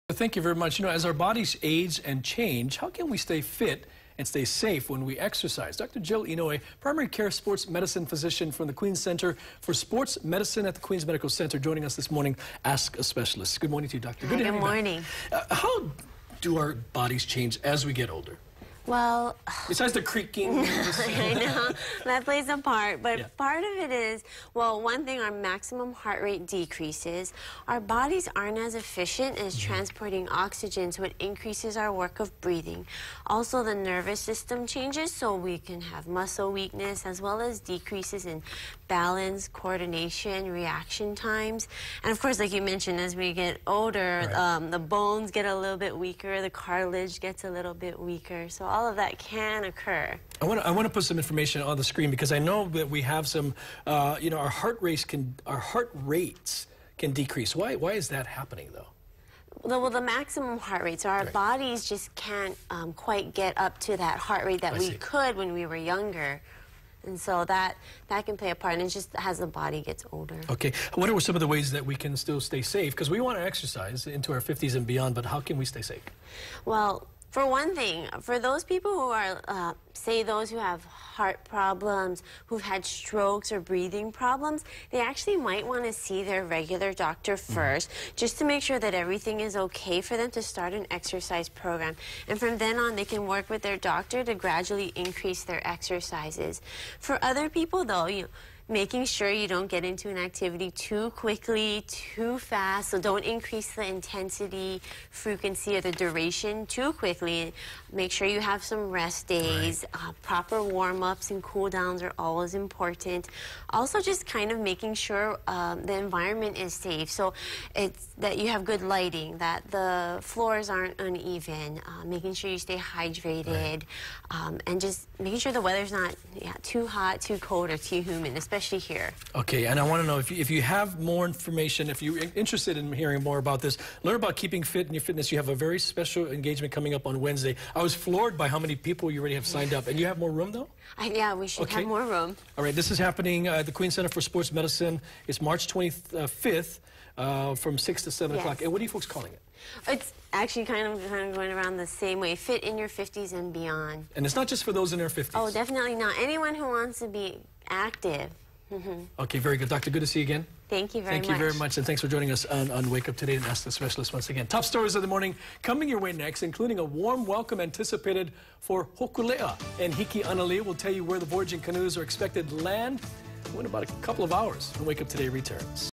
Ask a Specialist: Staying Fit after 50
Dr. Jill Inouye, primary care sports medicine physician at The Queen’s Center for Sports Medicine, at The Queen’s Medical Center, advises people how to stay fit after 50. To learn more about Keeping Fit in Your Fifties and Beyond, join Dr. Inouye and the experts from the Queen’s Center for Sports Medicine, including a physical therapist and registered dietitian, at a free Speaking of Health lecture at Queen’s – Punchbowl.  That’s on Wednesday, March 25, from 6 – 7 pm.